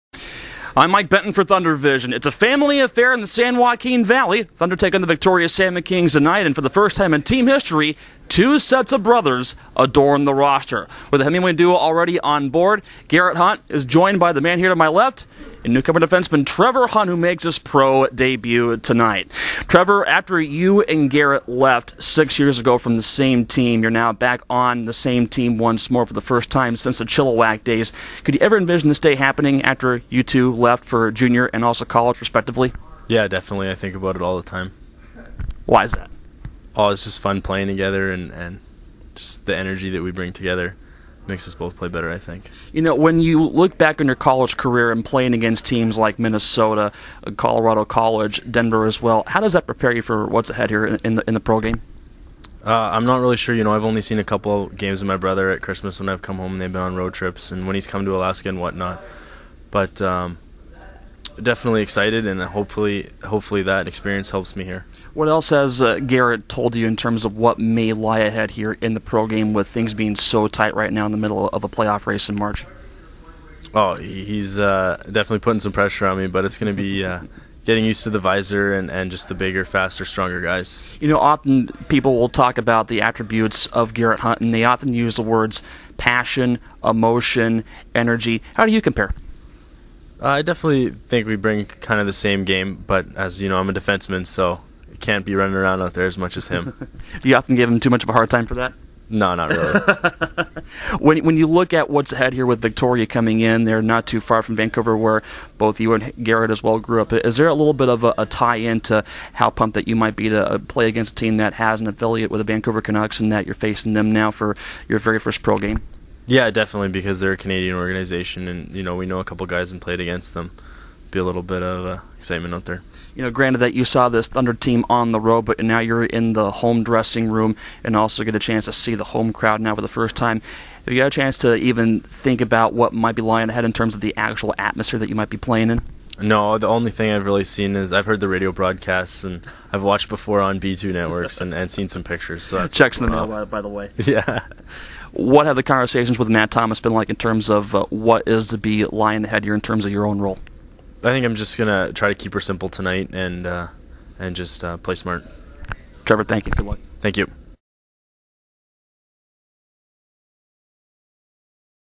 Pregame comments: 3-17-10 Trevor Hunt
New Thunder defenseman Trevor Hunt spoke with radio play-by-play announcer Mike Benton before the team's home game against the Victoria Salmon Kings on Wednesday, March 17, 2010 at Stockton Arena.